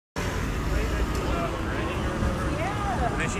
Whatisnewyork white dog standing on parking meter
a dog standing on a parking meter. a white dog stands on top of a parking meter/ parking payment booth